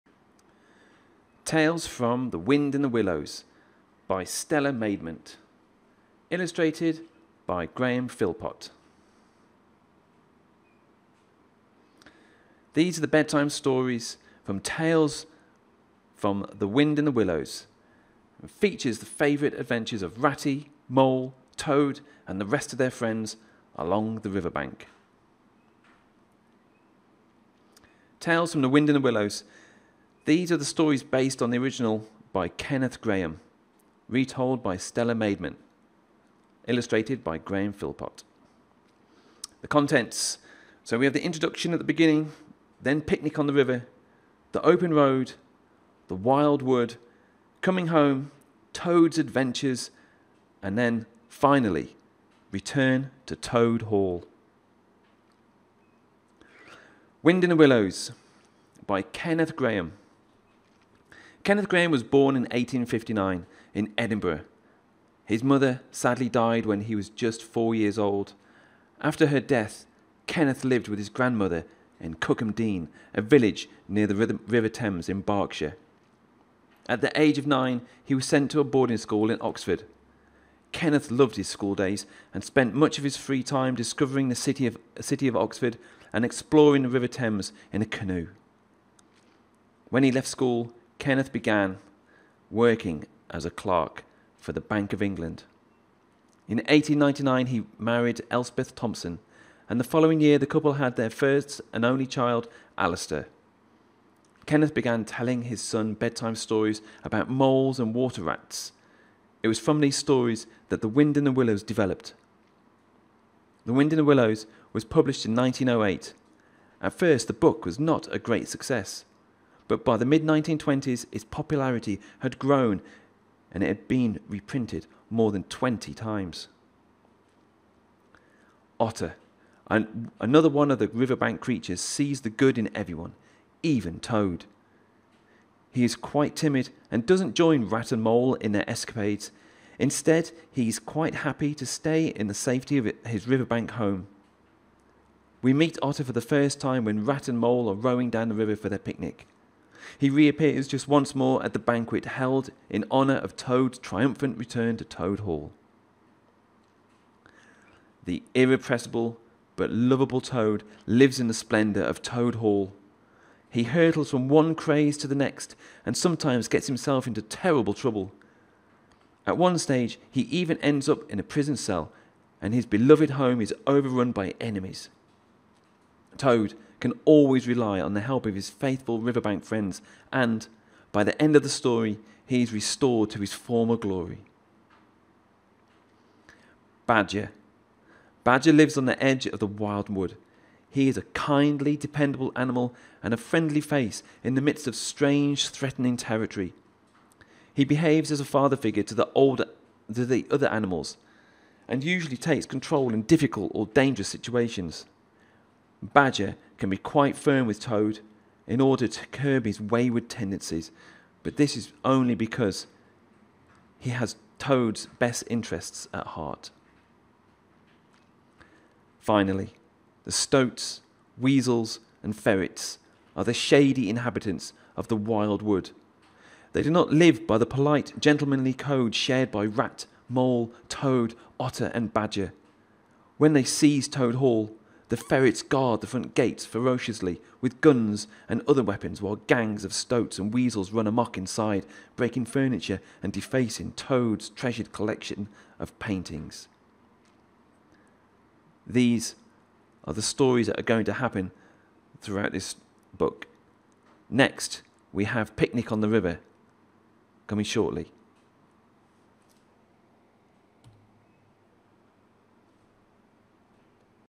The Wind in the Willows - Kenneth Grahame
The Wind in the Willows is a children's novel by Kenneth Grahame, first published in 1908. Alternately slow moving and fast-paced, it focuses on four anthropomorphised animals in a pastoral version of Edwardian England. The novel is notable for its mixture of mysticism, adventure, morality and camaraderie, and celebrated for its evocation of the nature of the Thames Valley.

Next Up, Mole Meets Rat

With the arrival of spring and fine weather outside, the good-natured Mole loses patience with spring cleaning. He flees his underground home, emerging to take in the air and ends up at the river, which he has never seen before. Here he meets Rat (a water vole), who at this time of year spends all his days in, on and close by the river. Rat takes Mole for a ride in his rowing boat. They get along well and spend many more days boating, with Rat teaching Mole the ways of the river.